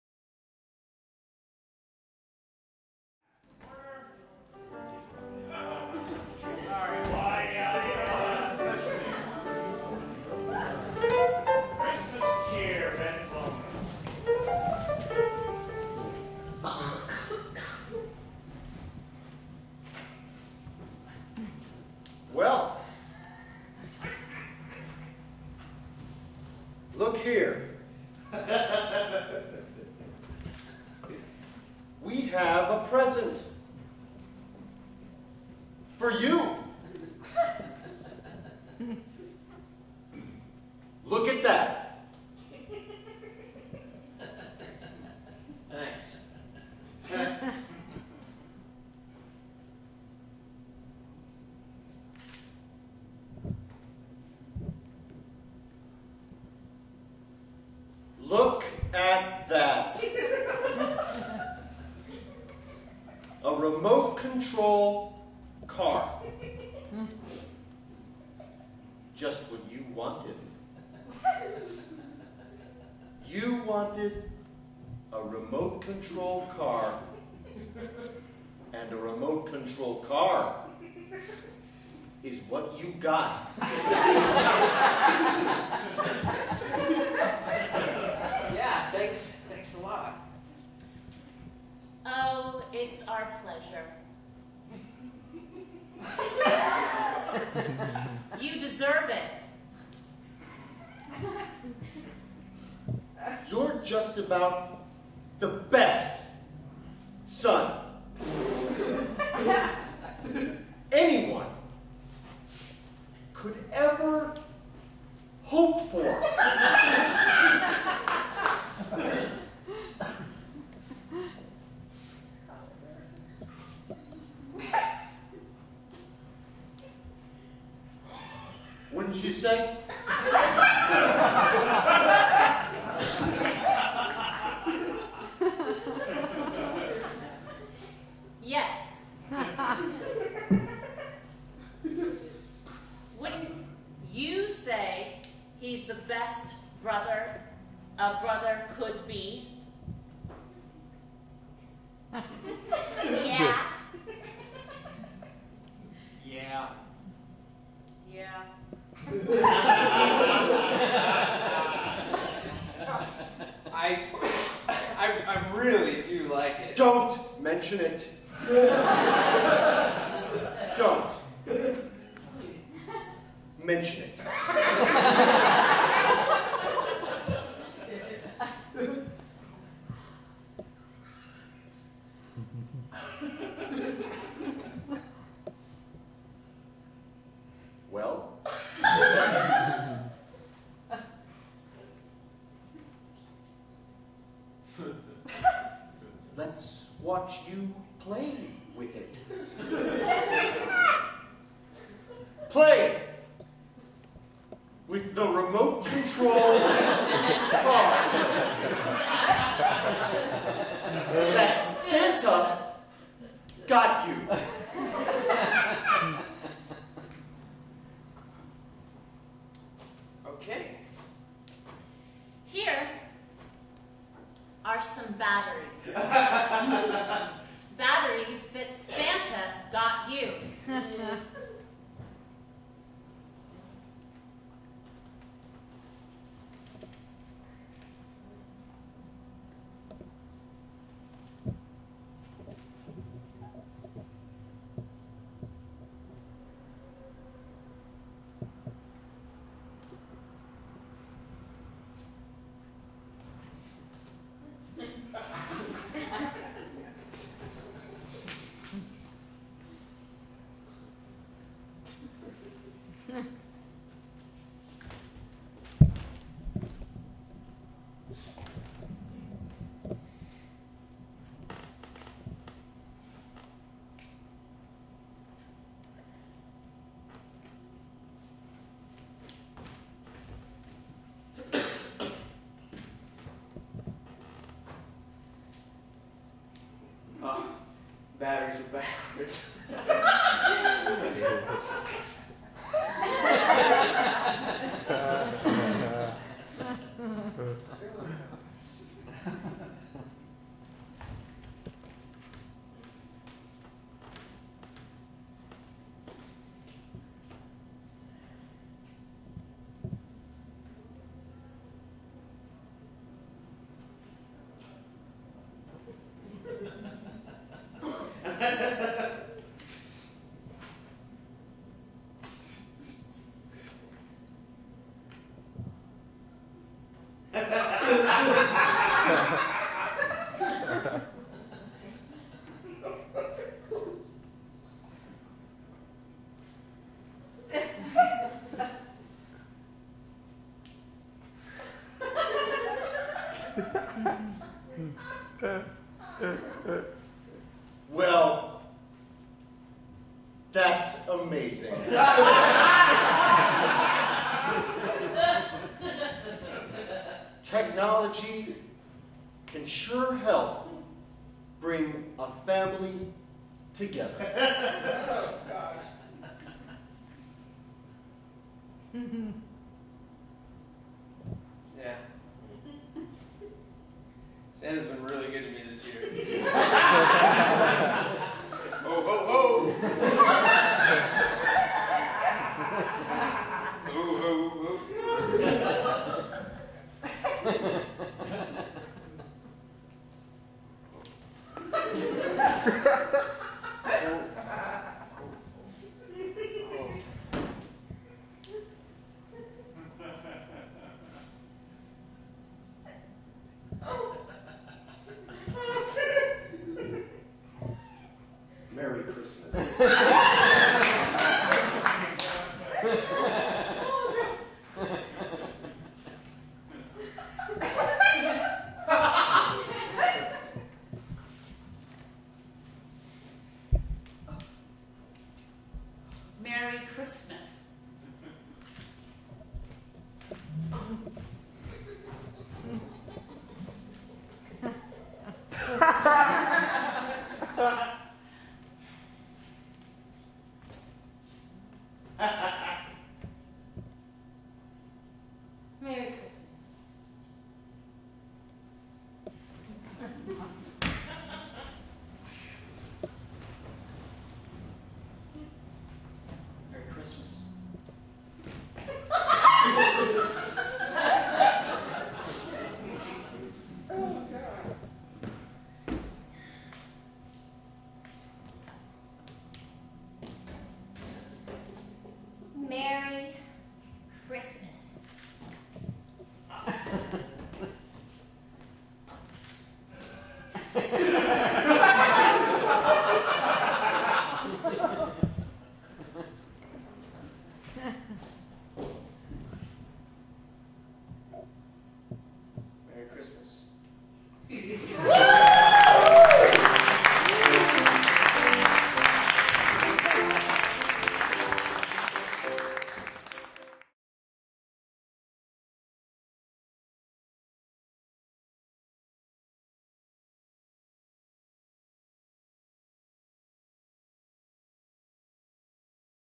Christmas Cheer by Ben Zolno - No Shame LA
Rehearsed 1 hour before the show and performed at No Shame Theatre Los Angeles May 19, 2006 in celebration of No Shame's 100th show; Starring Eric Johnson, Tenaya Cleveland, Ben Zolno, Megan Frances, Mike Rothschild and Julia Carpenter. For more videos and information about how to be involved with No Shame Theatre, please